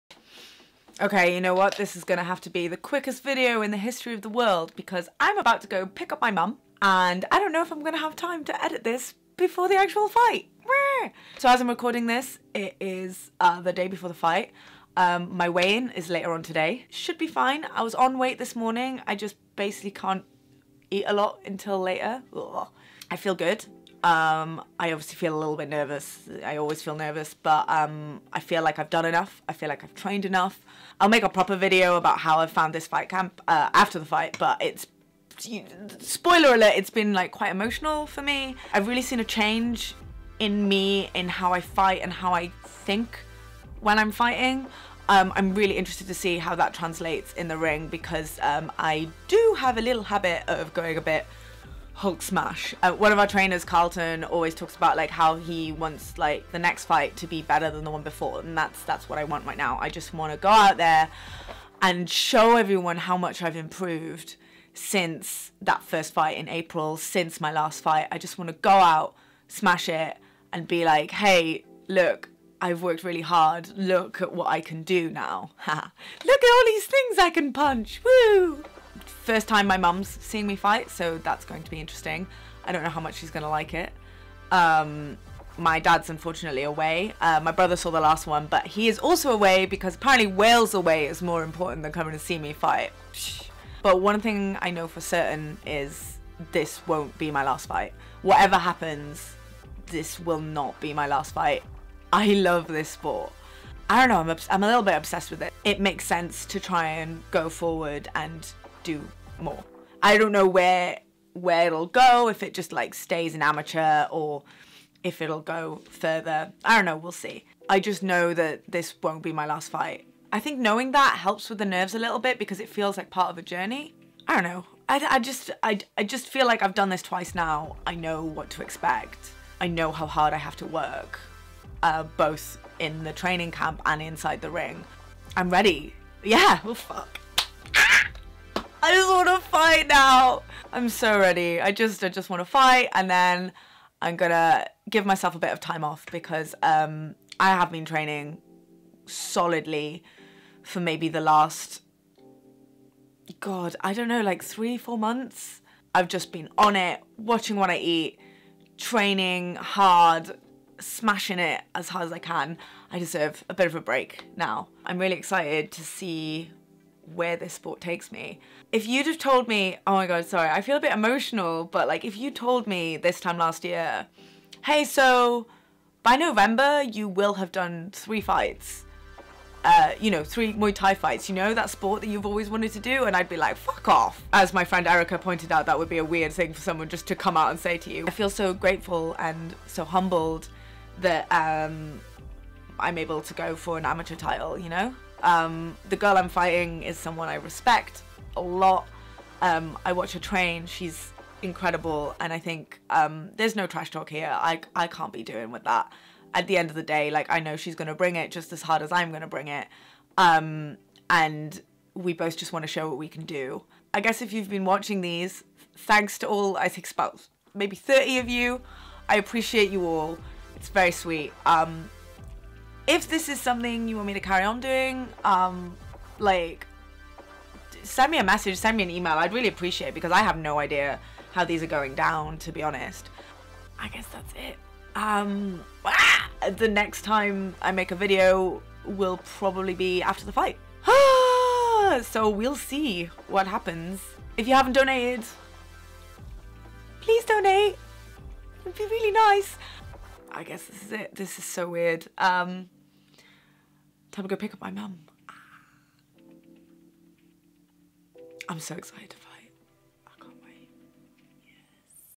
It's fight week aarghhhh | White Collar Fight Camp diary
MY FIGHT IS TOMORROW MY FIGHT IS TOMORROW

I’m raising money for the NSPCC by doing a white collar Thai boxing match with the White Collar Fight Club and Gymbox. It’s my third time in the ring, and it’s for a title, arghhhhhhhhh. I am terrified. I’ve decided to do a video diary because I love the sound of my own voice/want to raise even moooore money for the NSPCC.

You can find more updates about my fight journey on Instagram @elenacresci